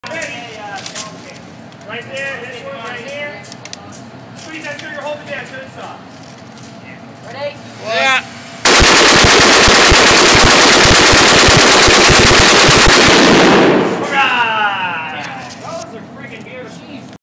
Fun at the gun range
Shooting some lead with some belt fed guns!